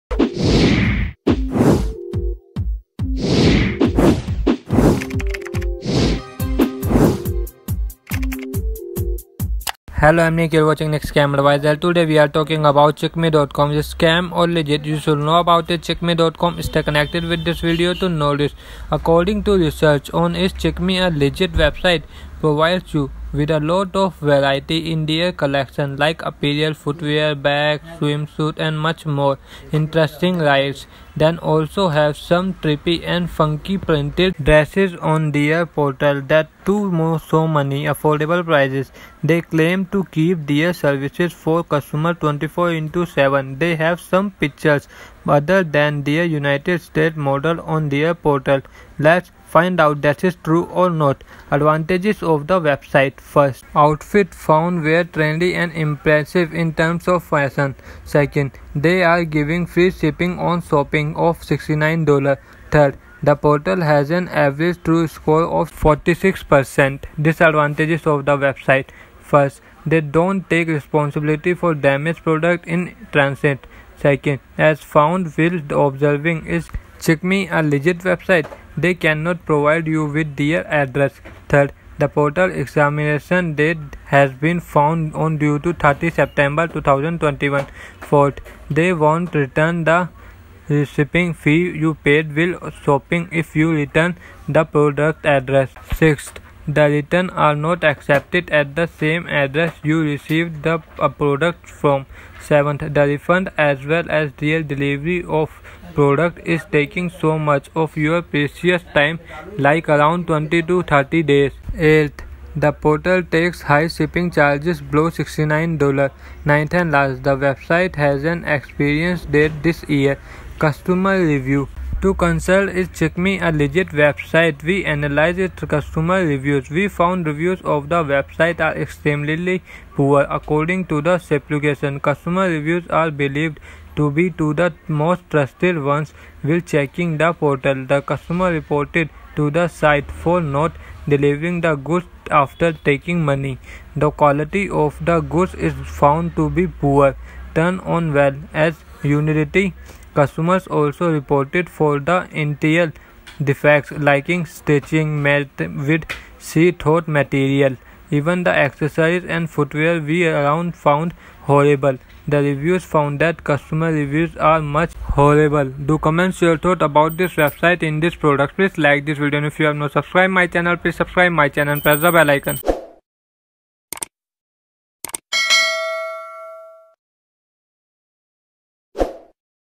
Chicme ! Chicme.Com Know if is it SCAM or LEGIT ? Chicme com reviews
As shown by the assessment on – Is Chicme A Legit Website gives you a huge load of variety in their arrangement like attire, footwear, sacks, swimsuits, and considerably more. Interesting right? By then in like manner have some trippy and shocking printed dresses on their passageway that too on such endless sensible expenses. They assurance to save their organization for customers the entire day. They have a couple of pictures other than their United States models on their passage.